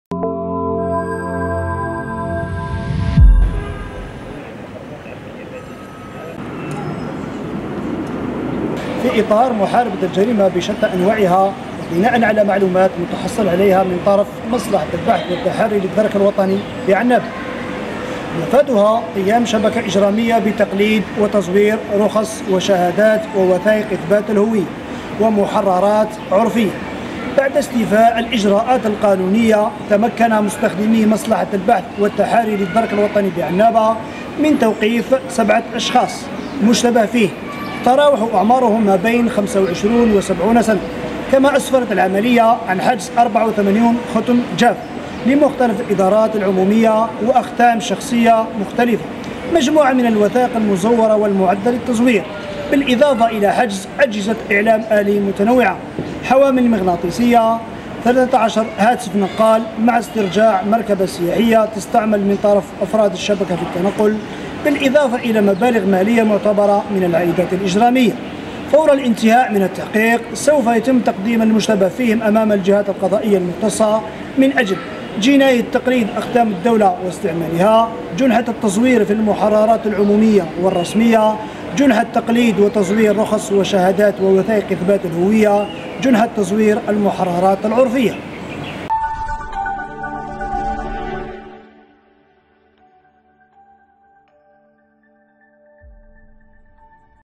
عنابة : توقيف 7 أشخاص متورطين في تقليد وتزوير وثائق رسمية ومحررات عرفية
عنابة (وأج) - تمكن أفراد الدرك الوطني بعنابة من توقيف سبعة مشتبه فيهم، متورطين في تقليد وتزوير رخص وشهادات ووثائق إثبات الهوية ومحررات عرفية، حسب ما أفاد الرائد سلوم عاشور، المكلف بالاتصال بالمجموعة الإقليمية للدرك  الوطني بالولاية.